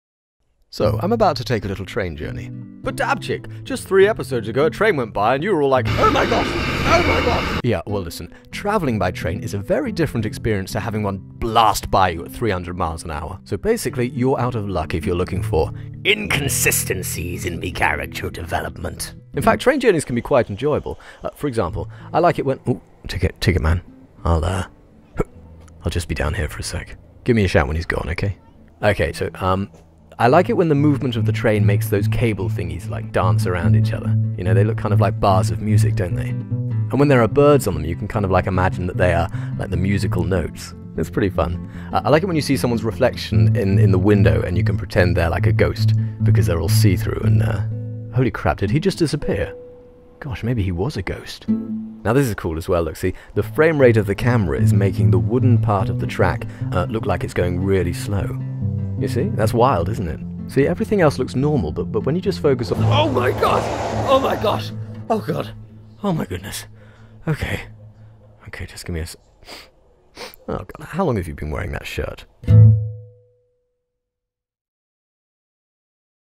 Dabchick ~31~ Train Journey
Dabchick takes a little train journey